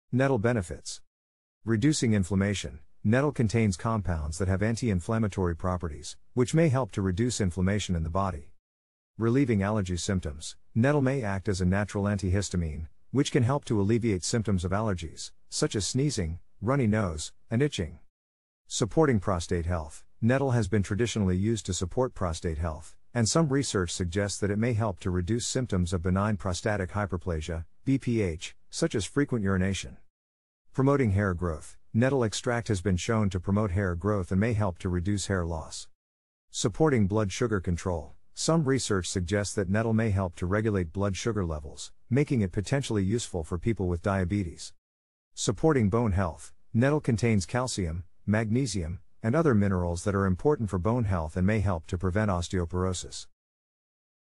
NETTLE BENEFITS
Nettle, also known as stinging nettle, is a plant that has been used for centuries for medicinal purposes. Here are some of the potential benefits of nettle:

Reducing inflammation: Nettle contains compounds that have anti-inflammatory properties, which may help to reduce inflammation in the body.

Relieving allergy symptoms: Nettle may act as a natural antihistamine, which can help to alleviate symptoms of allergies, such as sneezing, runny nose, and itching.

Supporting prostate health: Nettle has been traditionally used to support prostate health, and some research suggests that it may help to reduce symptoms of benign prostatic hyperplasia (BPH), such as frequent urination.

Promoting hair growth: Nettle extract has been shown to promote hair growth and may help to reduce hair loss.

Supporting blood sugar control: Some research suggests that nettle may help to regulate blood sugar levels, making it potentially useful for people with diabetes.

Supporting bone health: Nettle contains calcium, magnesium, and other minerals that are important for bone health and may help to prevent osteoporosis.